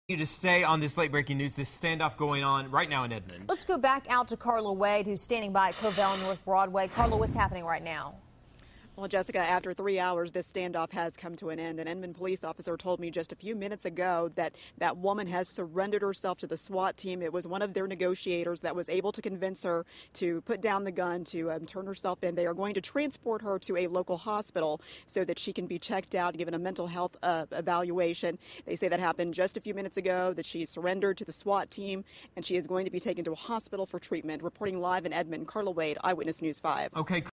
Police End Standoff In Edmond
Edmond Police able to bring standoff to peaceful conclusion.
Carla Wade reports.